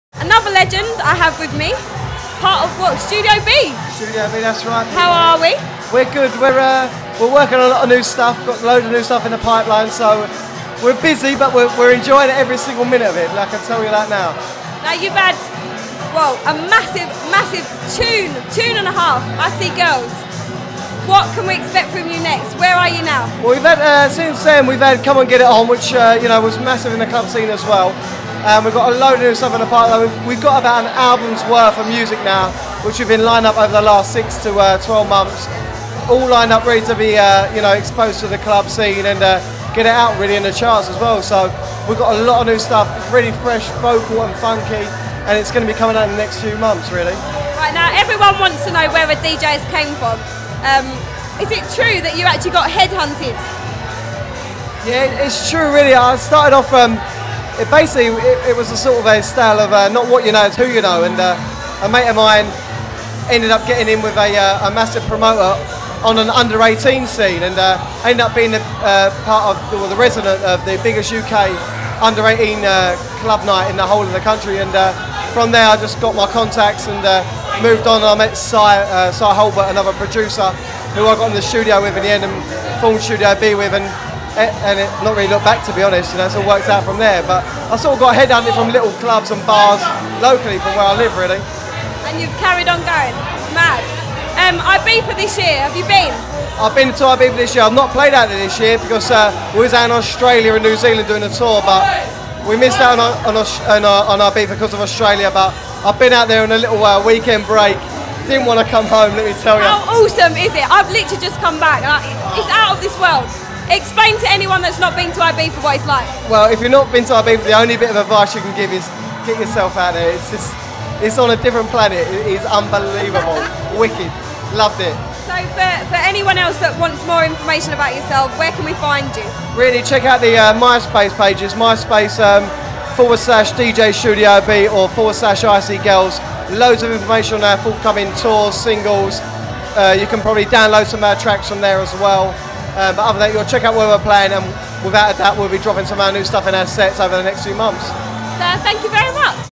studio B (Lewis Coleman)
Exclusive: Charlie from Kiss100 interviews Studio B @ The Drayman (Ibiza Reunion)